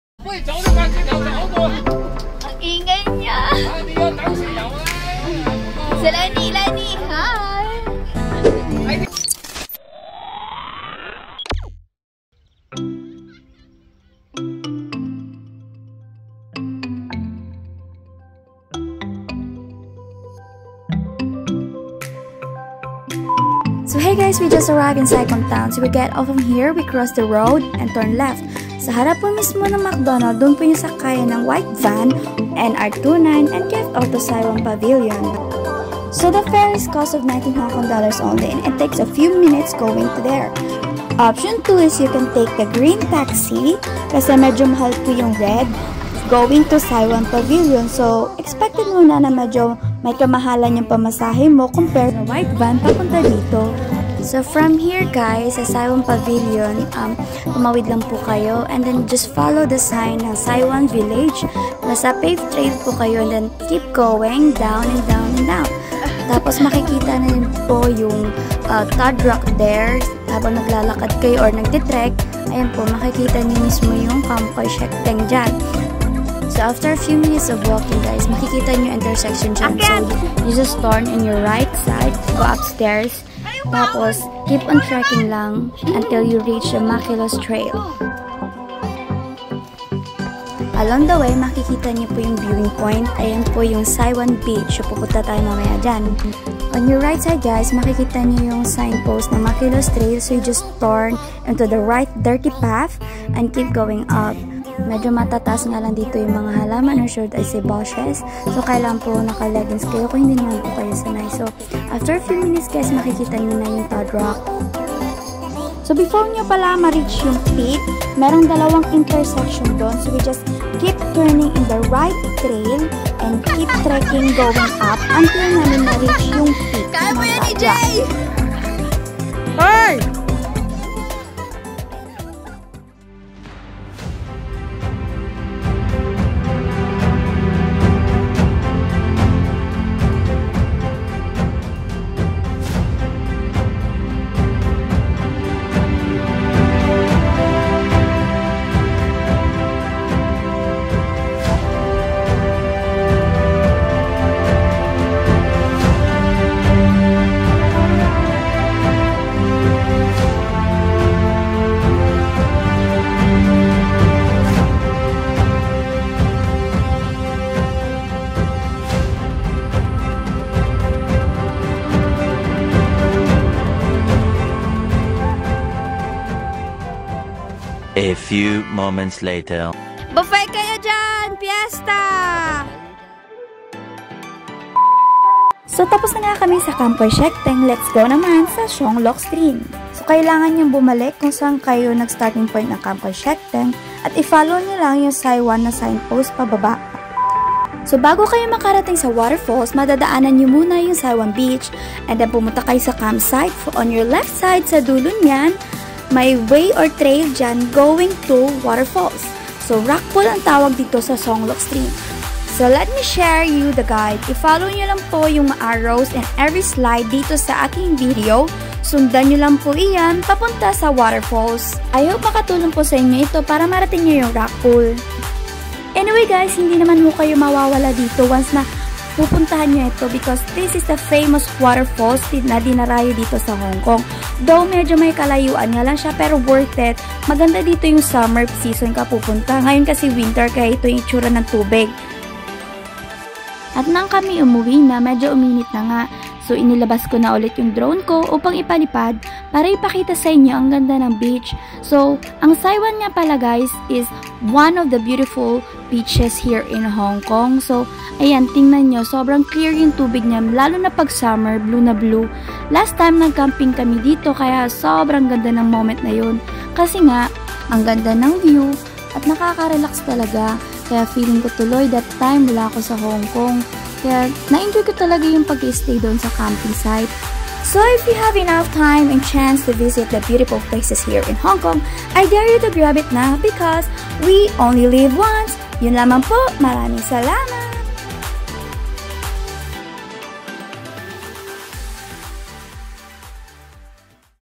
[Mini Vlog] Kam Kui Shek Teng ||Guide|| Easy Hike|| Rock Pool || Sai Wan Beach|| Sai Kung
***LeaveNoTrace

Getting to Sai Kung:
📍OPTION 1
▪️ From Diamond Hill  MTR Station ,Exit C2
▪️ Go to Bus Terminus & Take Bus no. 96R or 92
▪️Get off to Sai Kung Town Hall Bus Stop
▪️ and Walk going to McDonald..

📍OPTION 2
▪️Take MTR to Choi Hung Station,Exit C2 ▪️Take the Green Minibus 1A -Going to Sai Kung tell the Driver you will get off to McDonald.
  or Bus 92 Get off Sai Kung Town Hall Stop

📍OPTION 3
▪️From Mong Kok Station ,Exit E2
▪️Take Red minibus from 43-55 Dundas St, Mong Kok just near in Foot Locker..
▪️Don't forget to tell the Driver you will get off to Sai Kung McDonald.